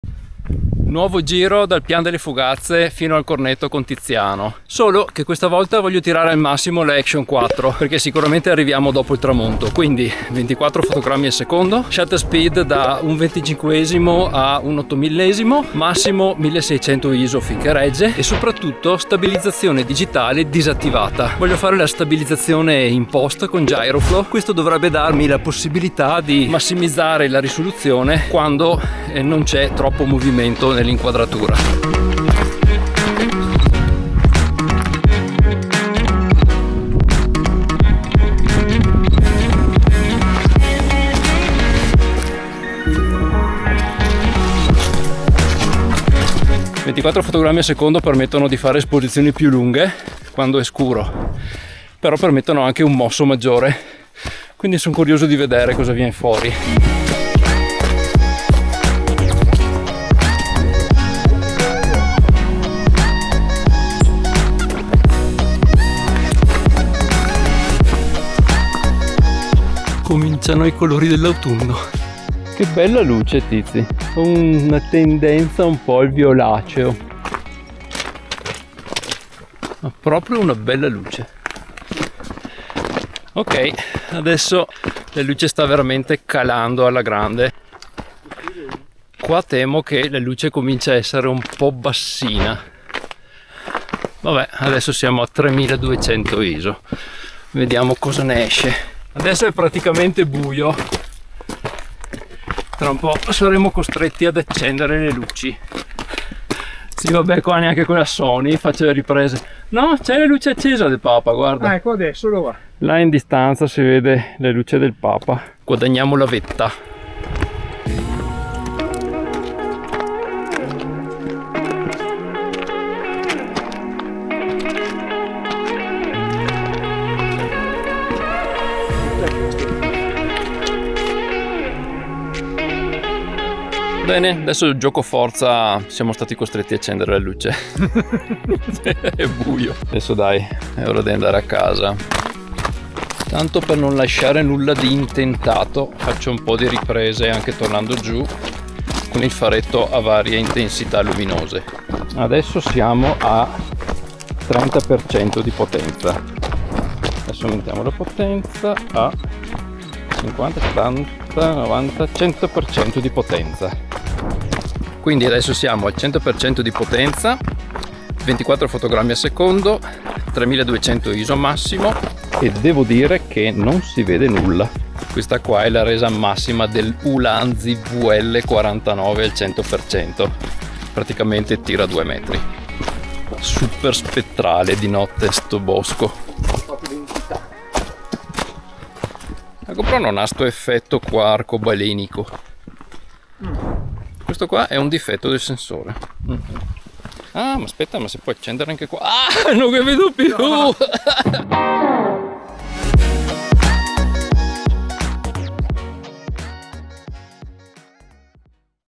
Action 4: Qualità Immagine in Notturna
Dopo i test dei precedenti video questa volta metto la DJI Action 4 sotto torchio partendo al tramonto e tornando a notte inoltrata. Uso inoltre Gyroflow per rosicare qualche pixel in più di risoluzione dove possibile e Davinci Resolve Temporal Denoiser unitamente a NeatVideo Denoiser per limitare il rumore agli alti ISO. Inaspettatamente scopro anche una limitazione nell'uso notturno.
Ne varrà la pena alla fine?
Scrivete le vostre impressioni nei commenti sotto!